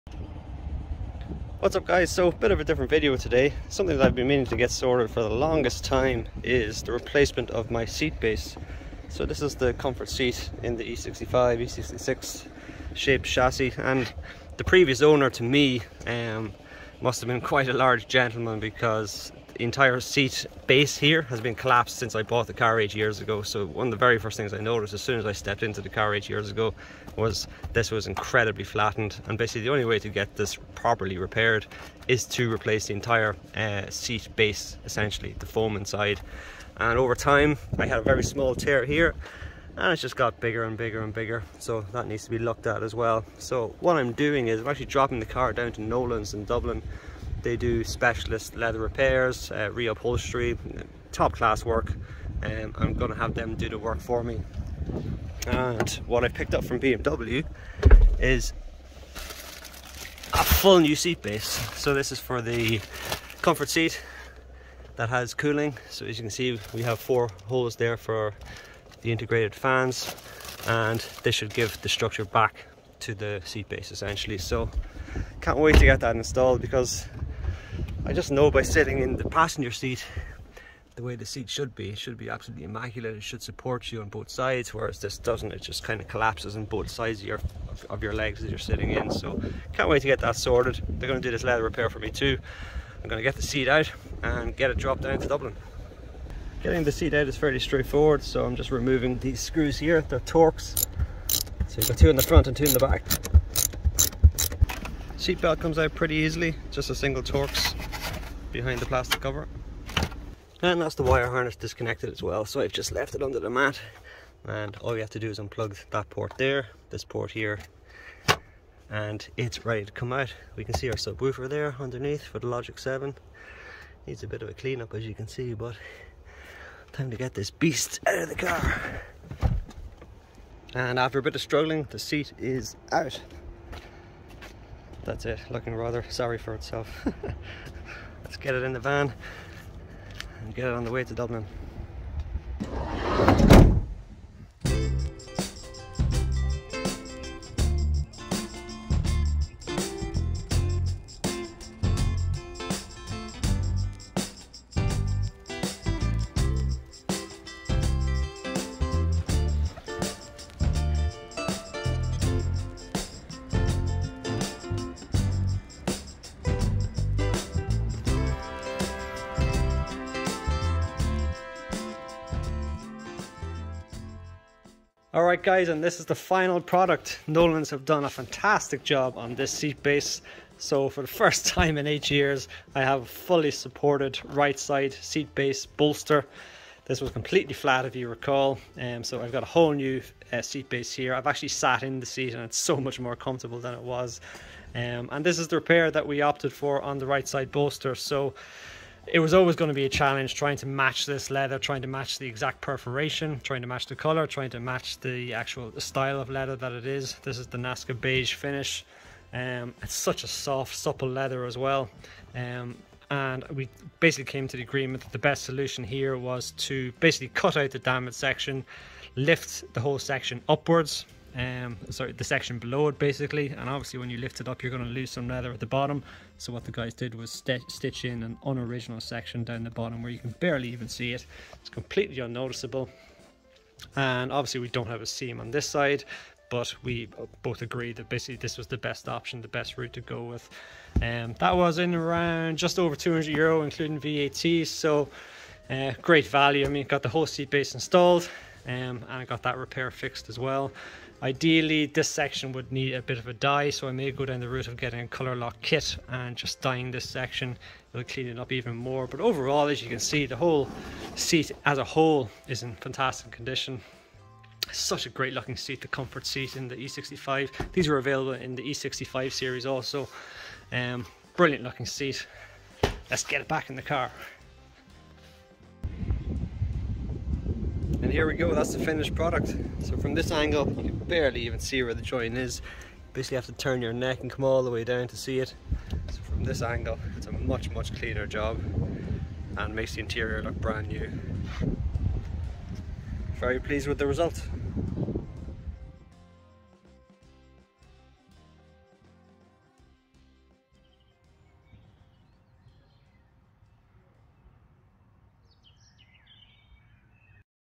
BMW E66 740Li Repair Series - Comfort Seat Refurbishment
In today's episode, I travel to Dublin with the driver side comfort seat to have the seat foam base replaced and a tear in the side bolster fixed.